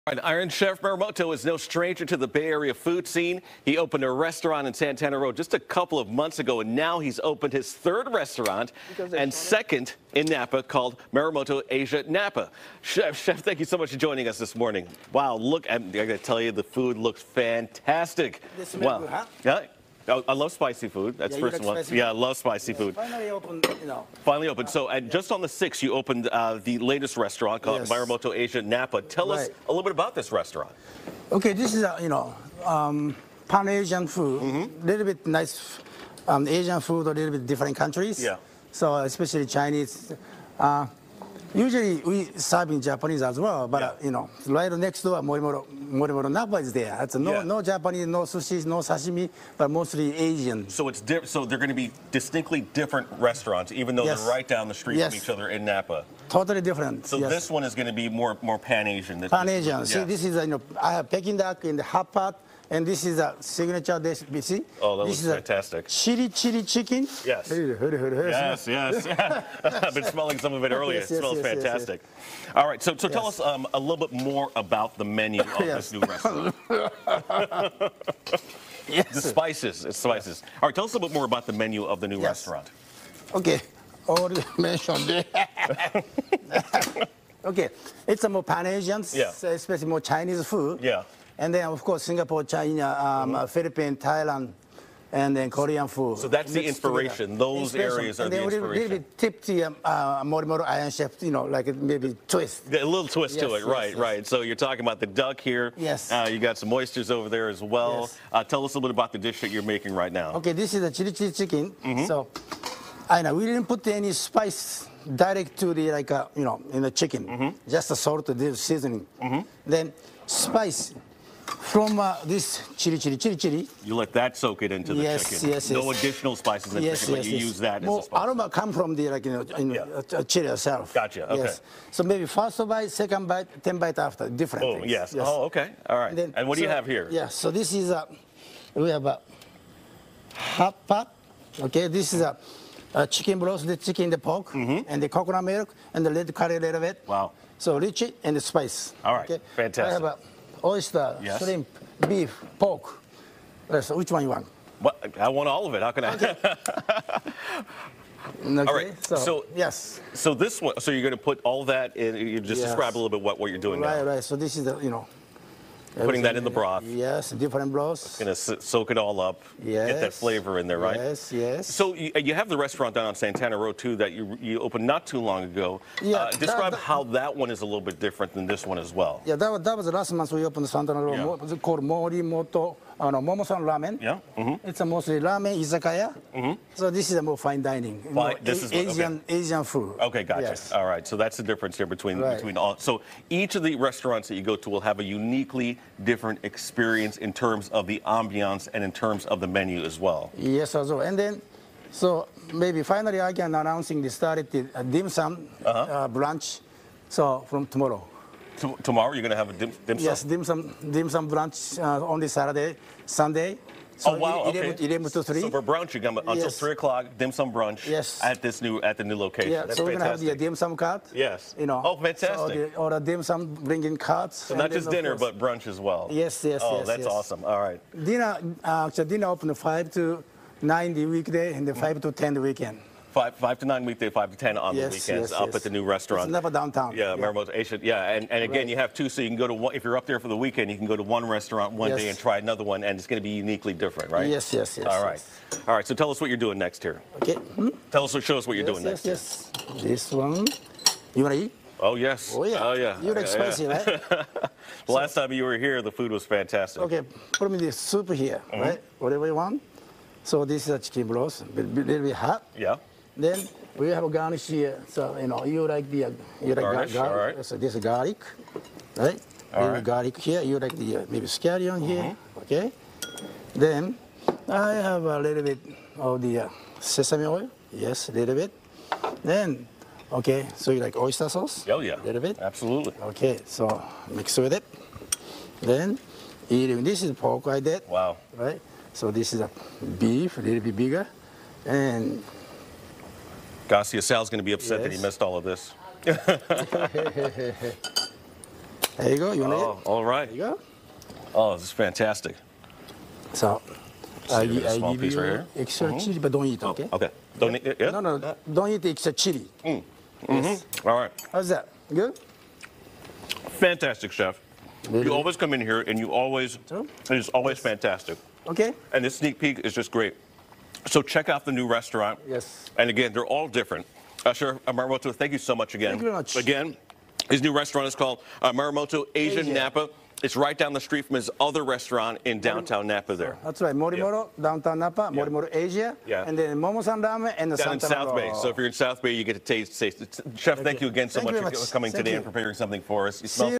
Iron Chef Morimoto's Hot Pot!
Iron Chef Masaharu Morimoto stops by to showcase his version of a hot pot that can be enjoyed at his latest restaurant venture, Morimoto Asia Napa.